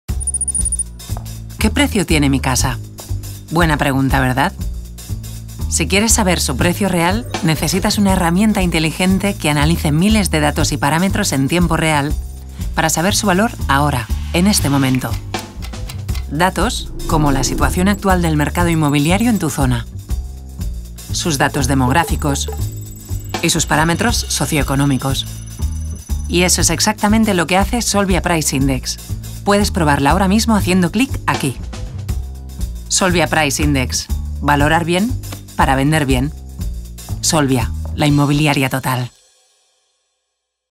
Solvia Price Index®
Valorar bien tu casa es el primer paso para venderla bien. Prueba la nueva herramienta inteligente de Solvia para clientes y no clientes